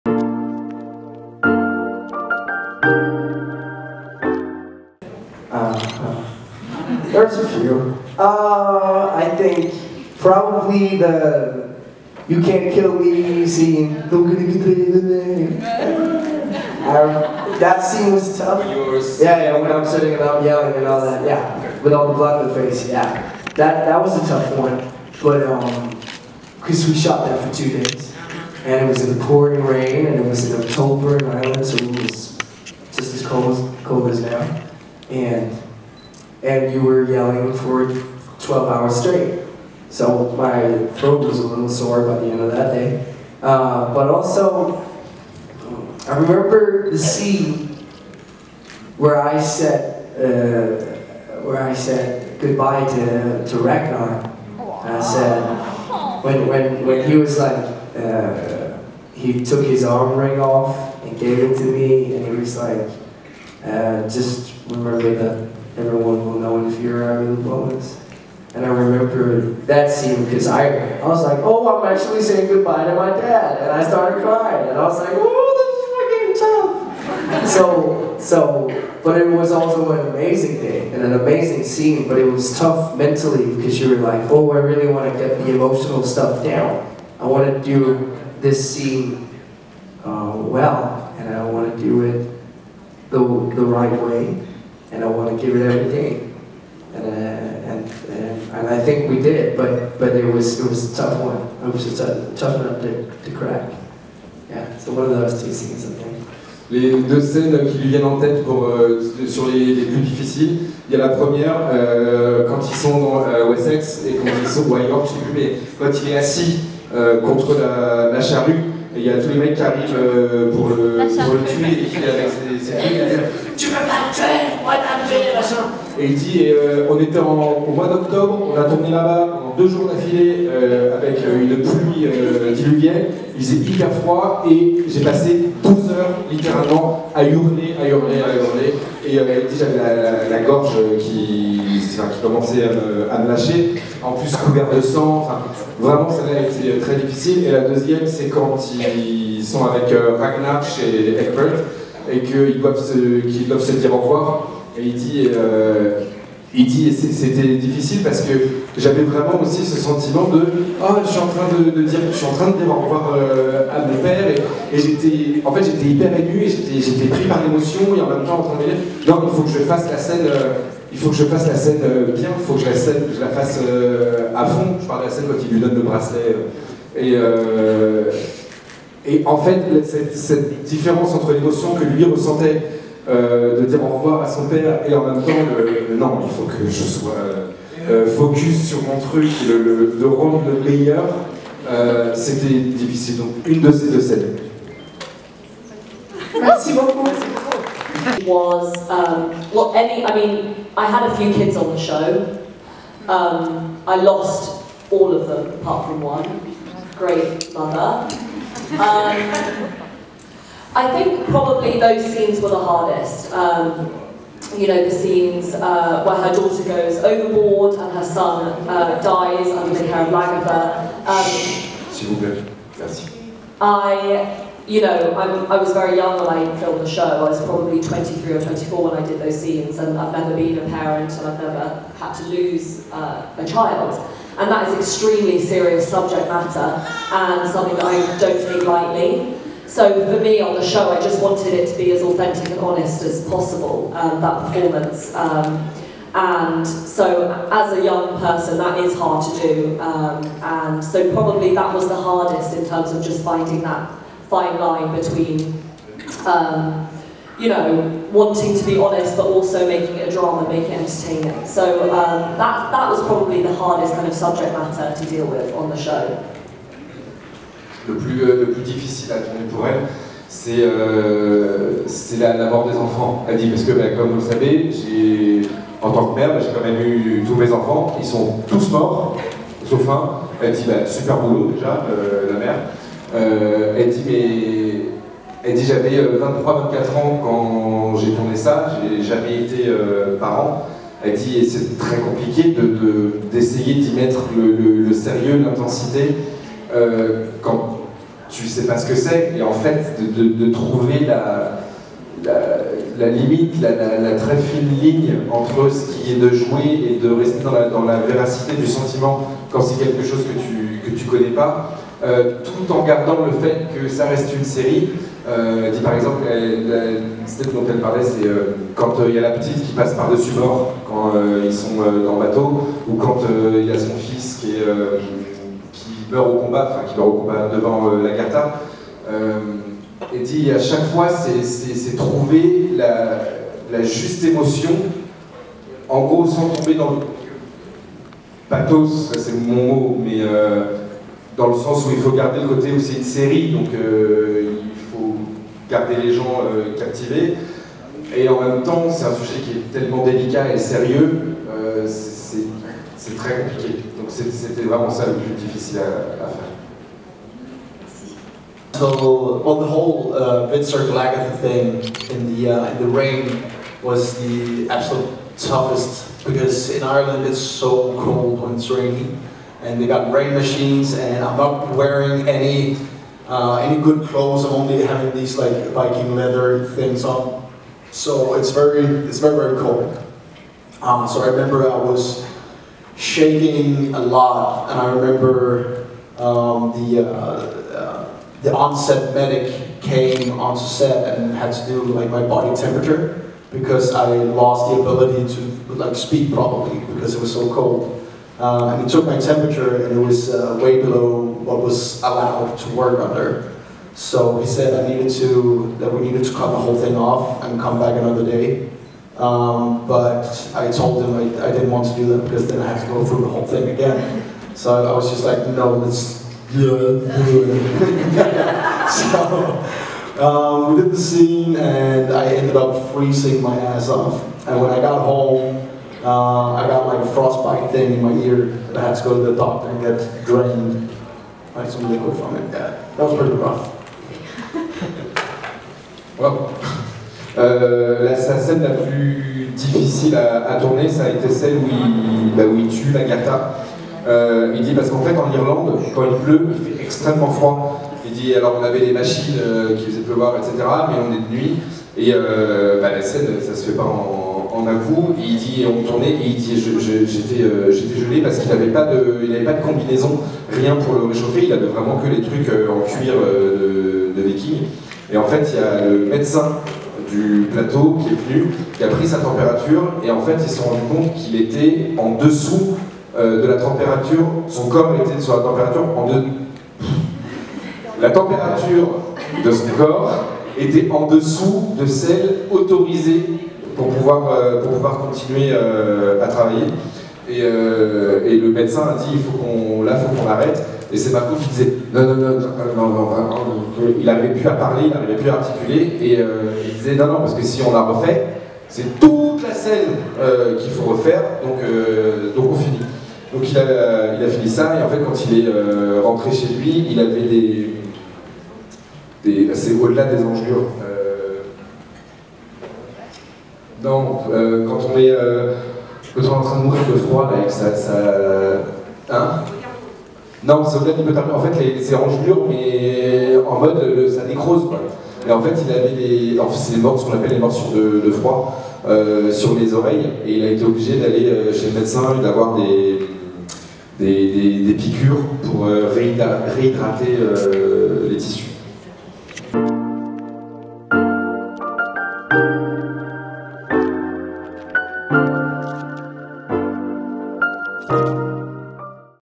Alex Høgh Andersen, Georgia Hirst and Marco Ilsø talk about their hardest scene to shoot in Vikings.
At the FKTP, Alex Høgh Andersen (Ivar), Georgia Hirst (Torvi) and Marco Ilsø (Hvitserk) talked about their hardest scene to shoot in vikings.
FrConventions is a media about French fan events.